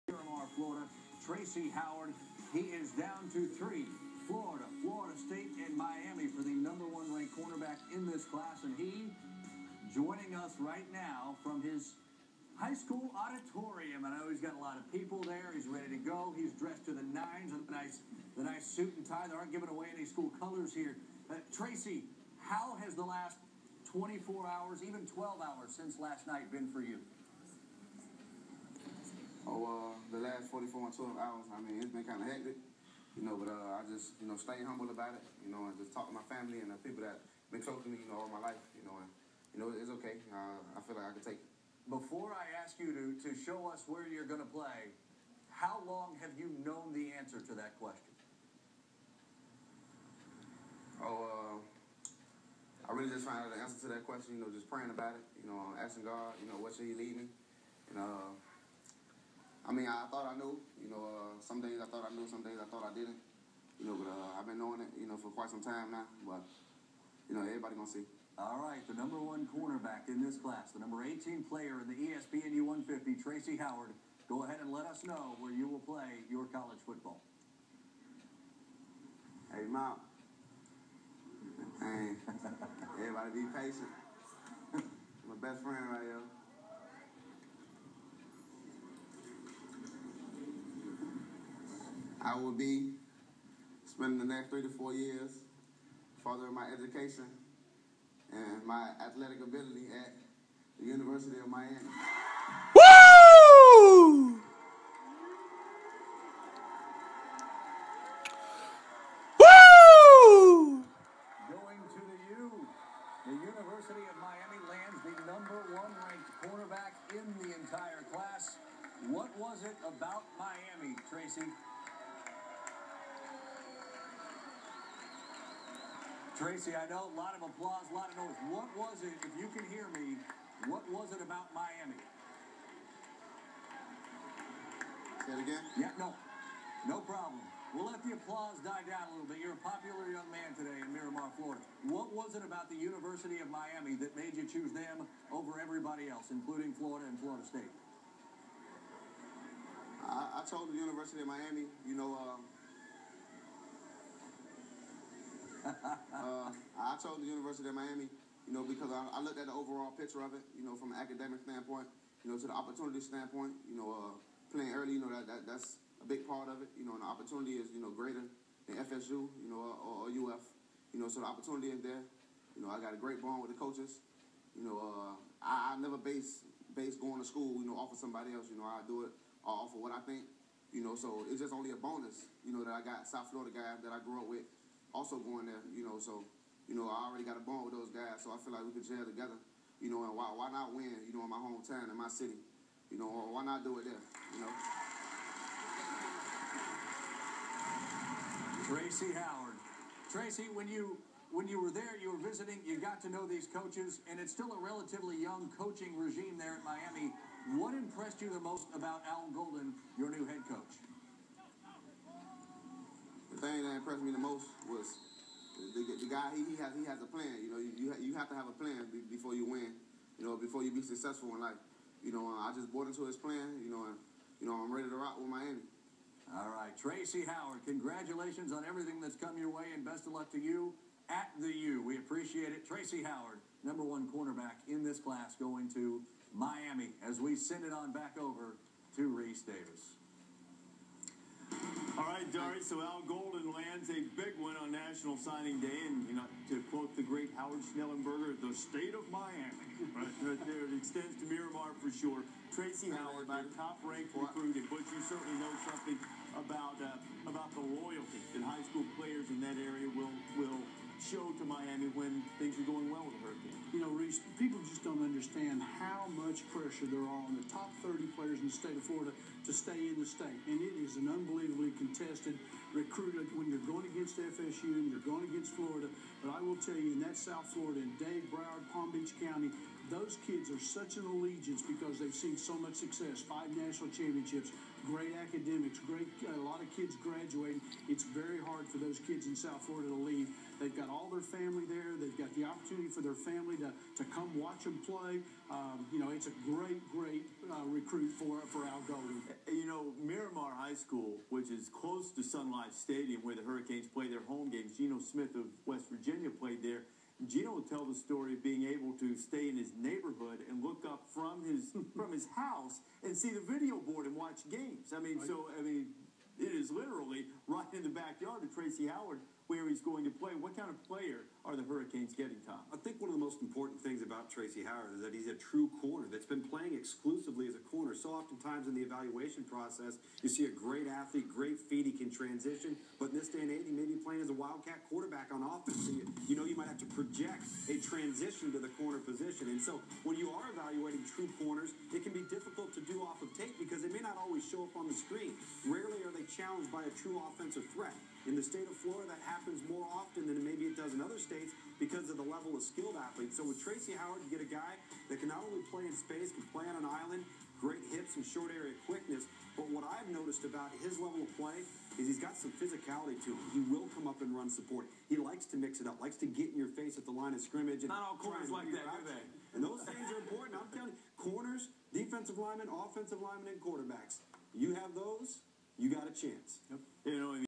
Tracy Howard picks THEU
Miami Hurricanes land the #1 CB in the country!!